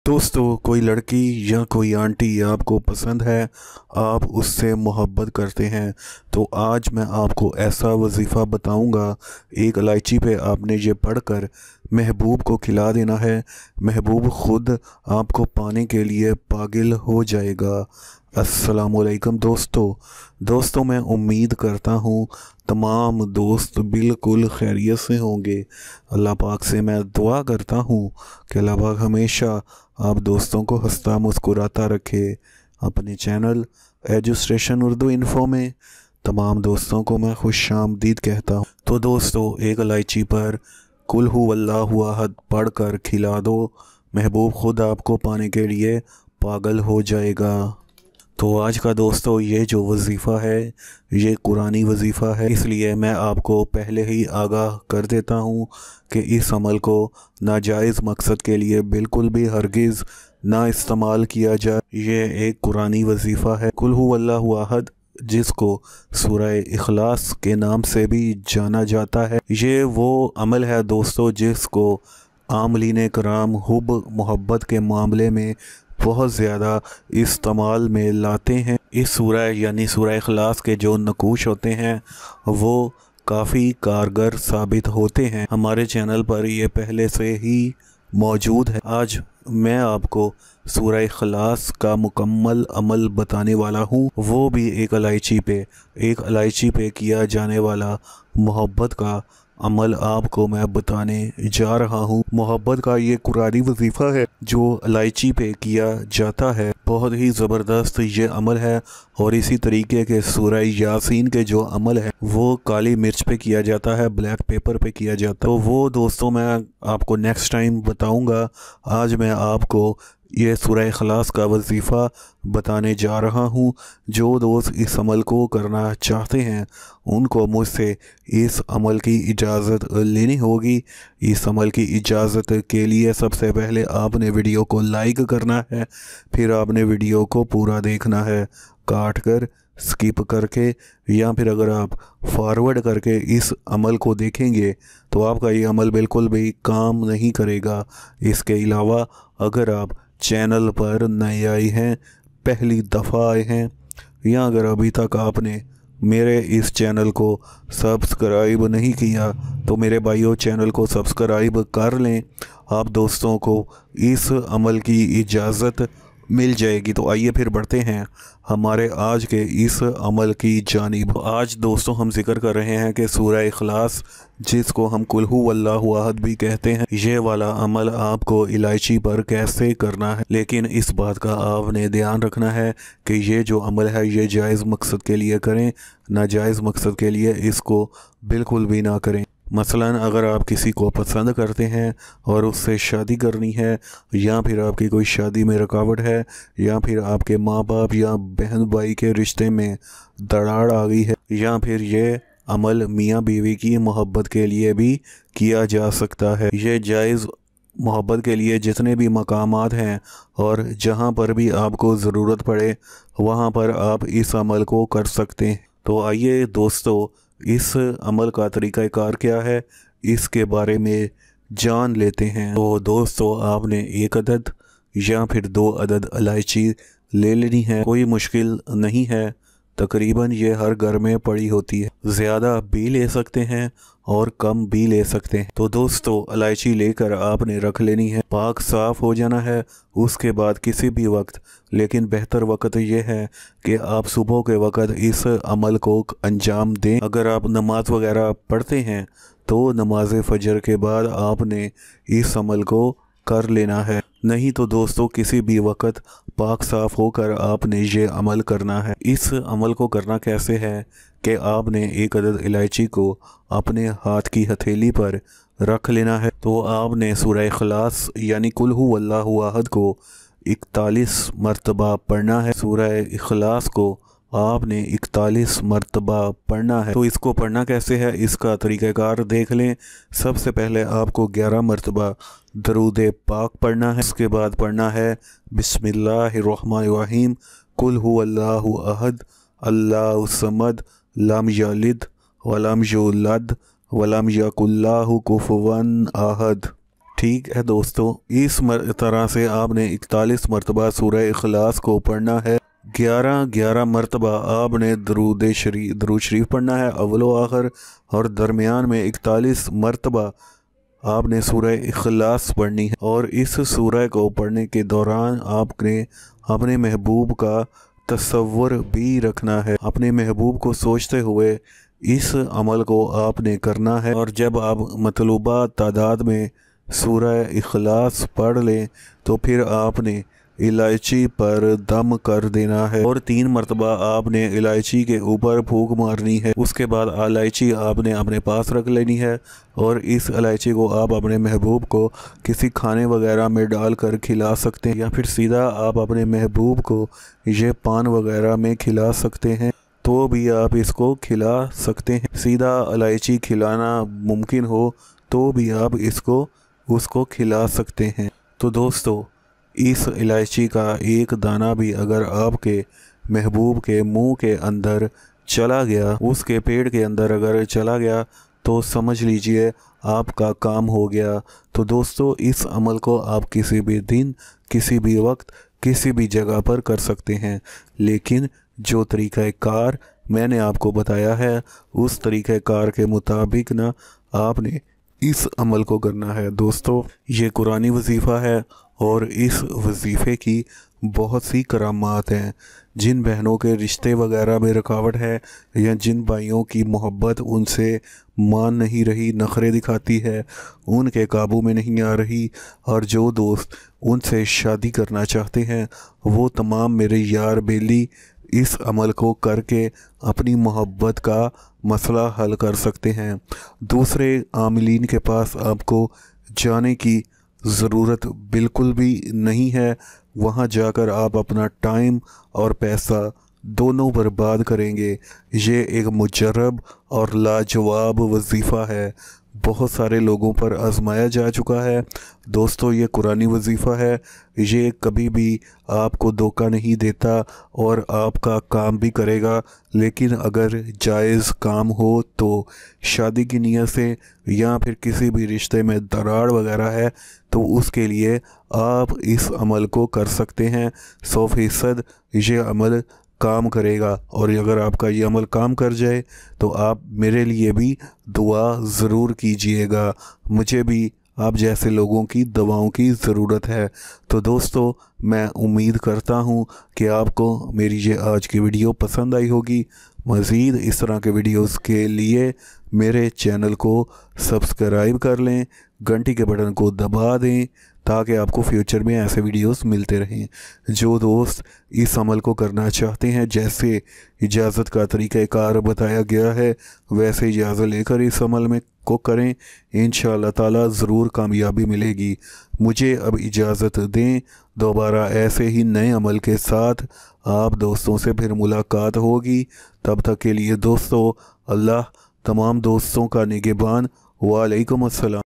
Mohabbat ka khas amal-Mohababt ka special amal-Love ka amal-Edustation Urdu Info
Is video me mohabbat ka khas amal dia gya hia agar ap ko koi pasand hai ya kisi se pyar karte hain aur us se shadi karne ke ley yeh amal kar sakte hain love ka yeh amal ap ko ap ka pyar kare gi